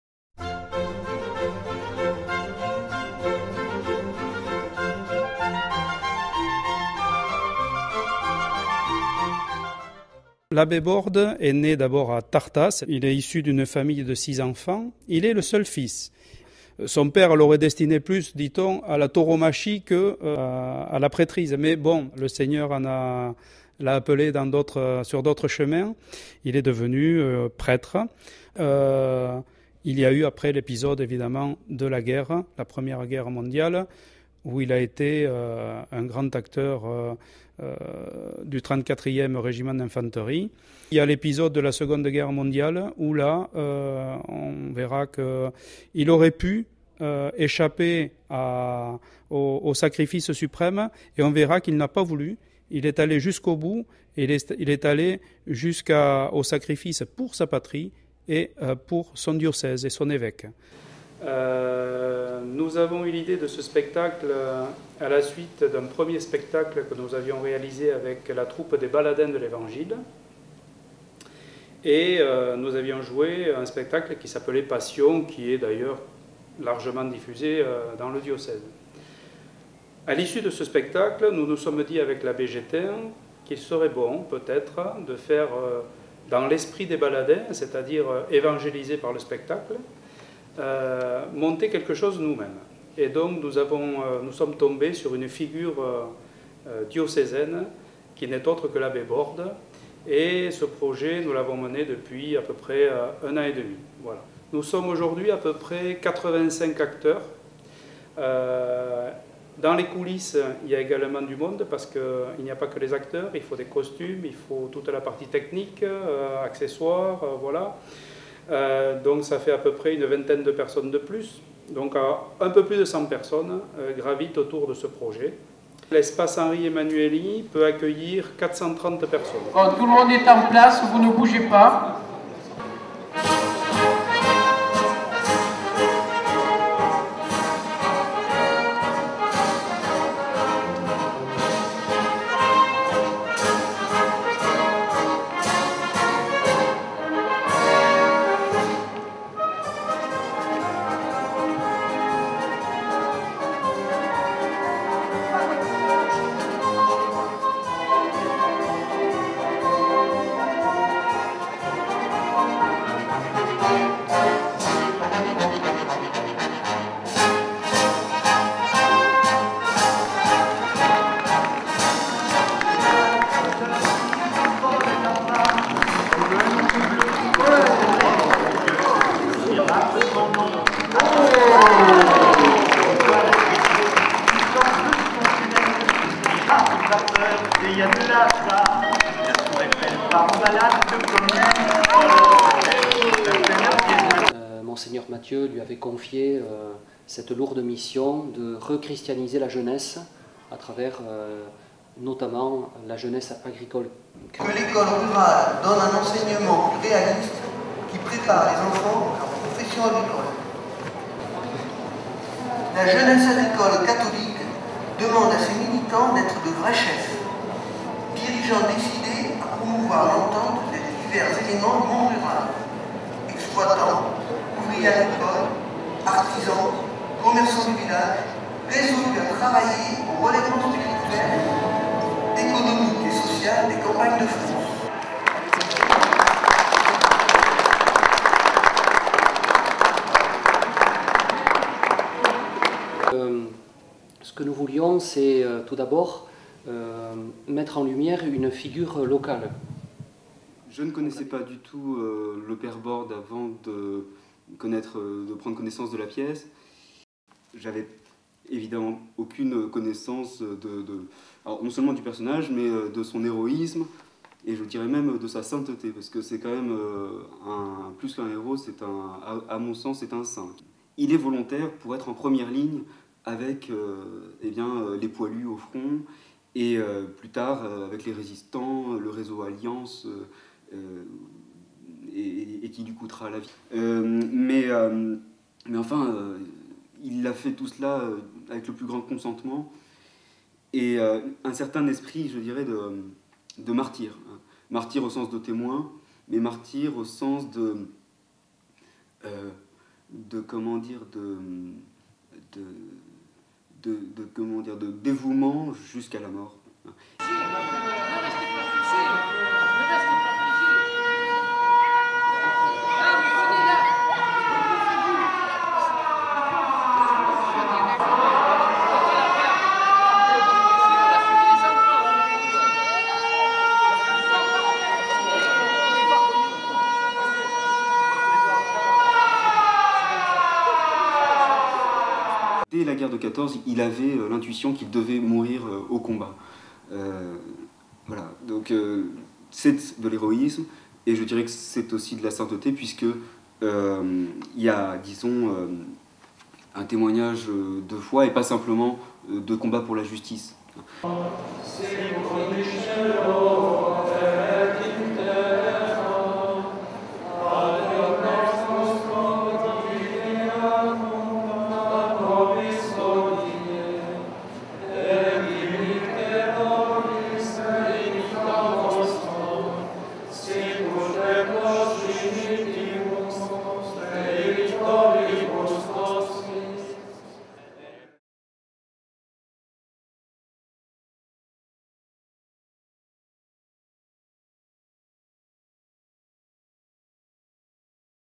Présentation du spectacle de l'Abbé Bordes - 6 avril 2019 - Mugron
Parole@dour s'est rendue aux répétitions du spectacle de l'Abbé Bordes programmé à Mugron le 6 avril prochain.
Découvrez la présentation de ce spectacle unique !